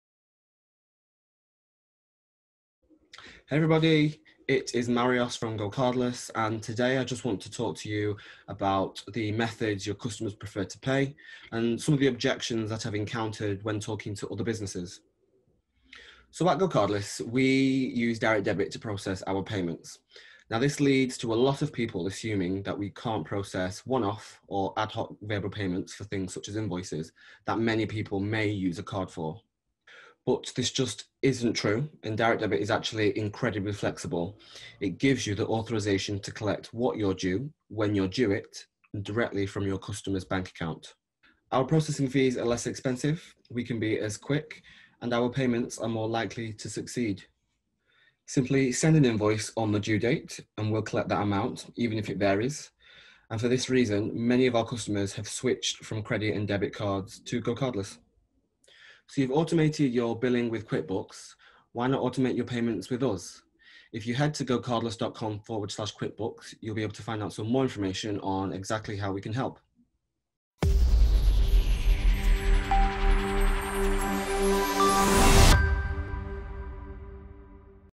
Better than credit and debit cards? | GoCardless for QuickBooks
Did you know that GoCardless can be used for one-off, adhoc or variable payments?

In this video, Marios explains why GoCardless is a better option for collecting payments compared to debit or credit cards.